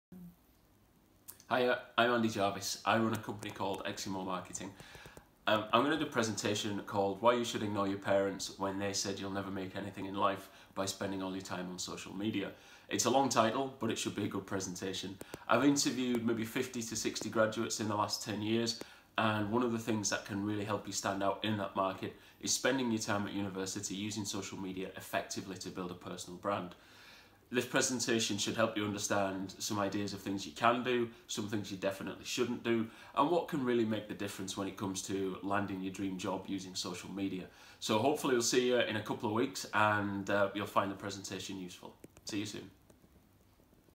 Andi Jarvis introducing his Welcome Week talk on social media
Why you should ignore your parents when they said you'd never make anything of your life by spending all your time on social media?
Even if your parents don't say that you need to pay attention. Social media offers an opportunity and a threat and Andi will tell you why and how in his Welcome Week talk.